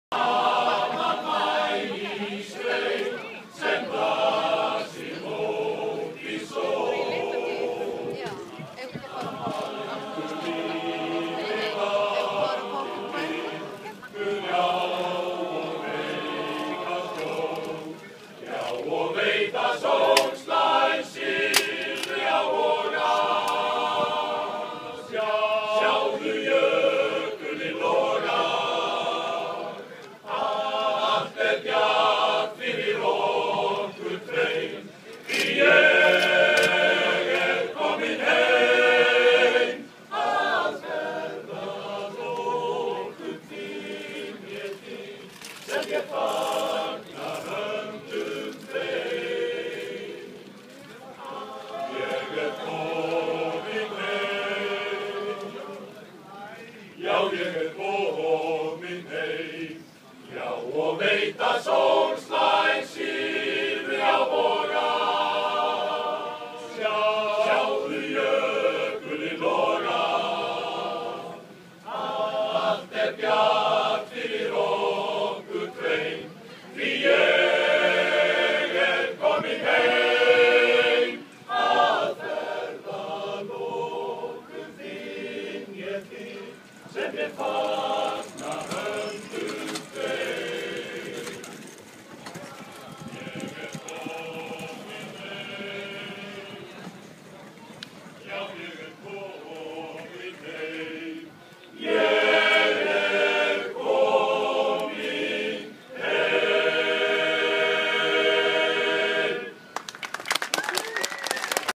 Reykjavik Bacon Festival Men's Choir
Icelandic Bacon Singers perform at the 3rd Annual Reykjavik Bacon Festival.